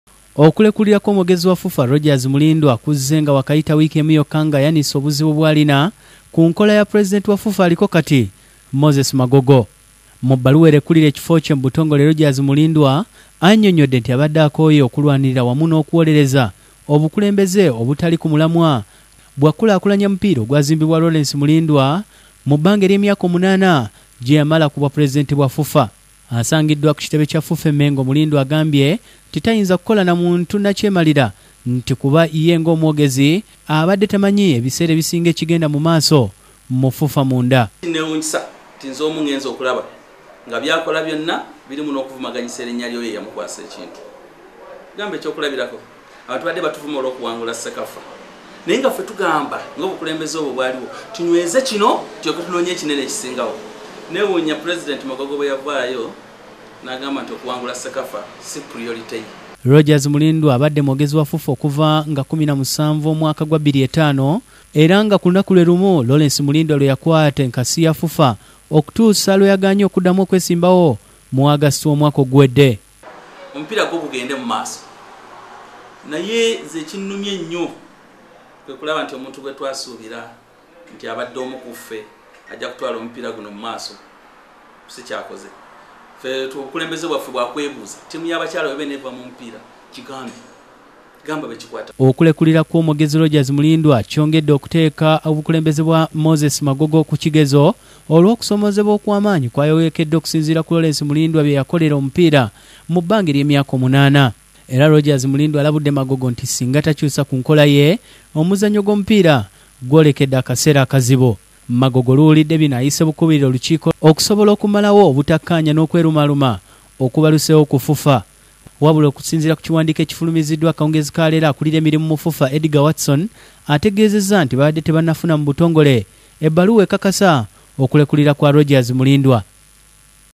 Abadde omwogezi wa FUFA Rogers Mulindwa alekulidde
'Magogo alina okukyusa, sikola na muntu atawulirizza balala'